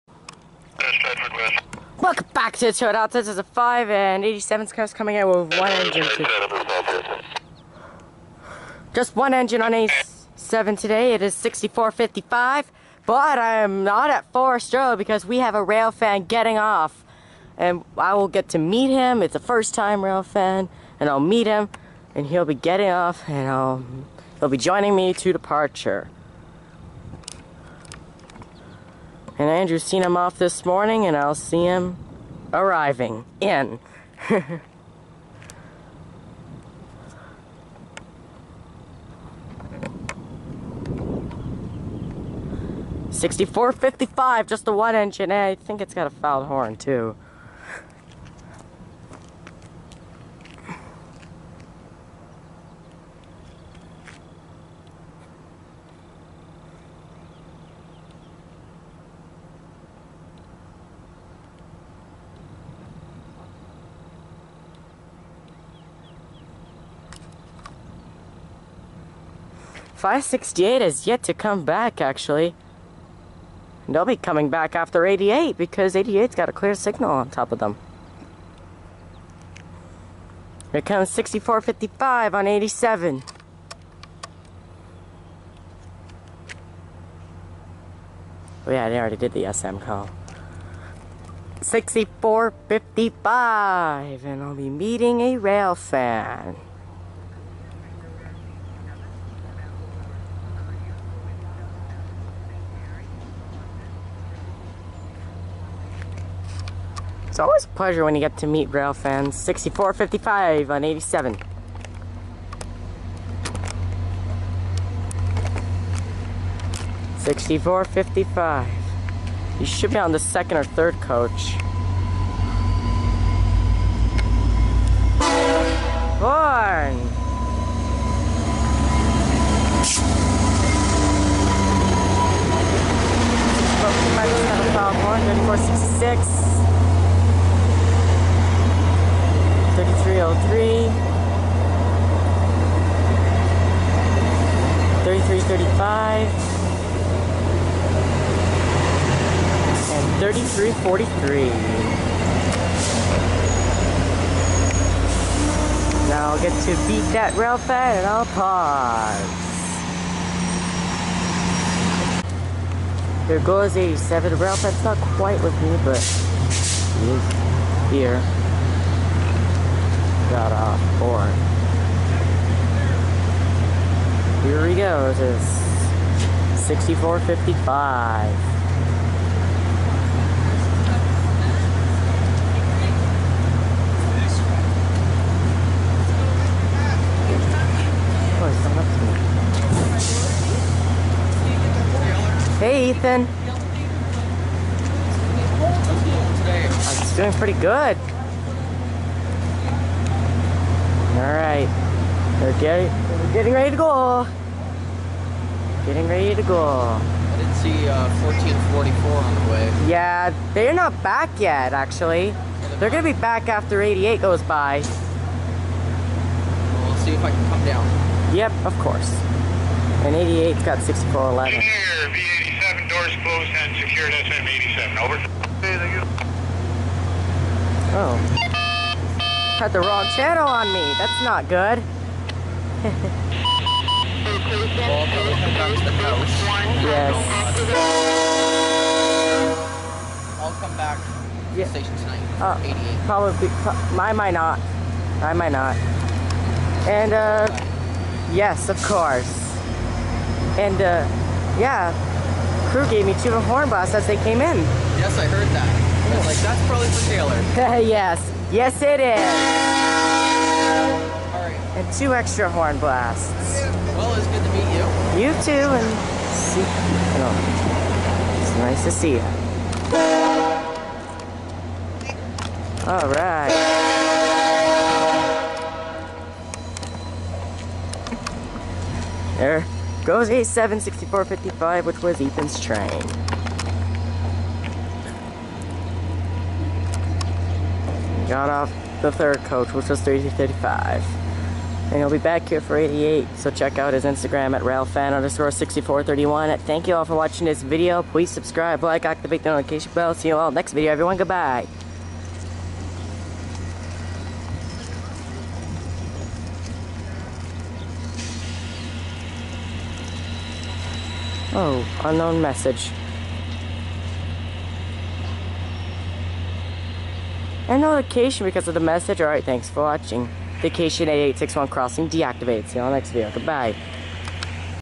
40 year, railfan getting off! VIA 87 July 16, 2019
Locomotive Number(s): 6455
Location: Stratford Station
Sub: Guelph
Milepost: 88.5
Route: Toronto-Sarnia